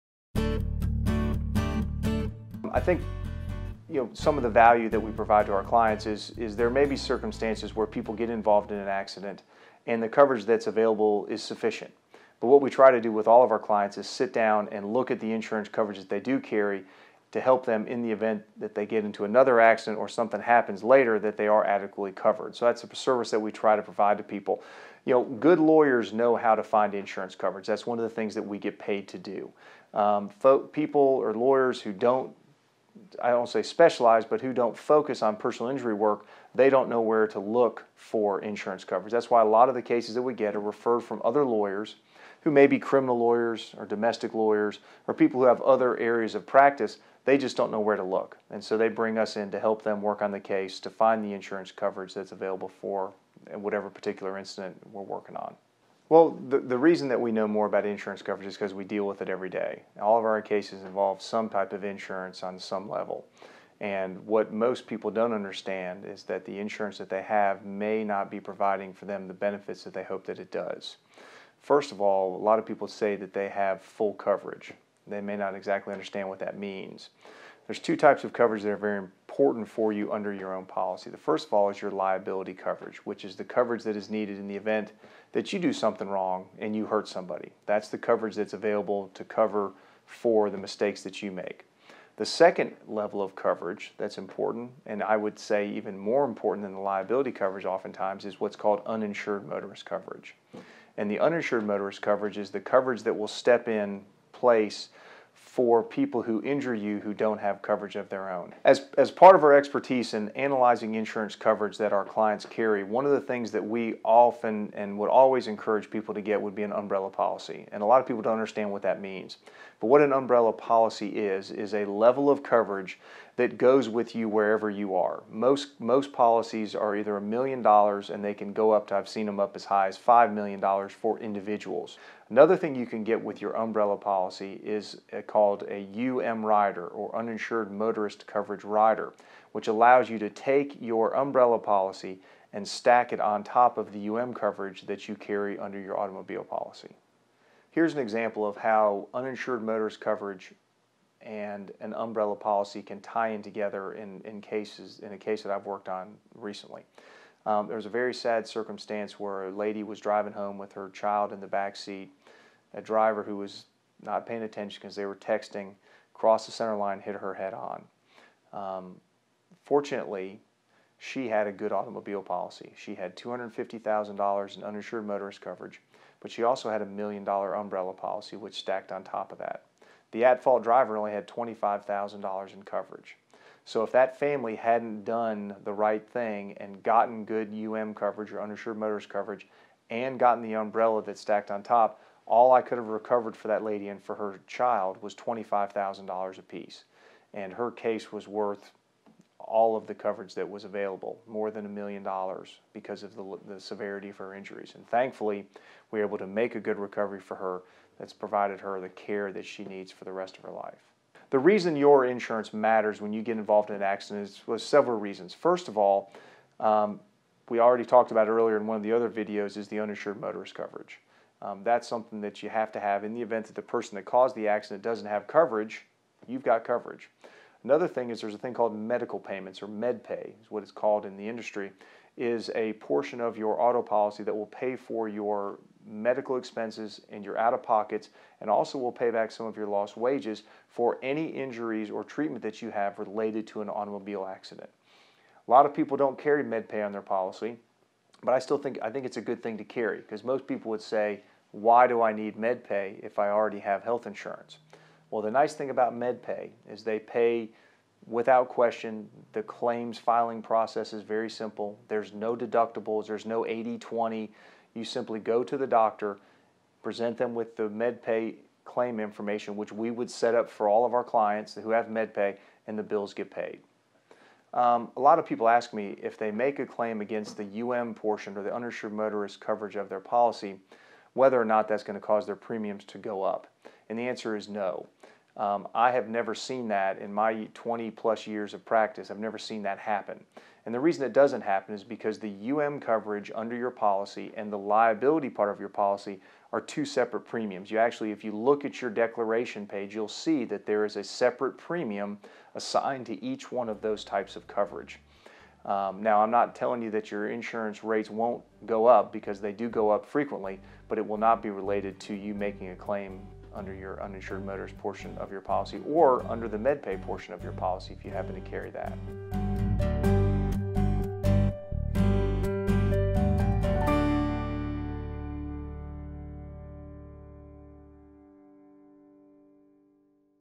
Helpful Facts About Automobile Insurance - What Coverage Should You Have?
Attorney Andrew Jones of Marietta, Georgia offers insight to automobile insurance coverage from a legal perspective.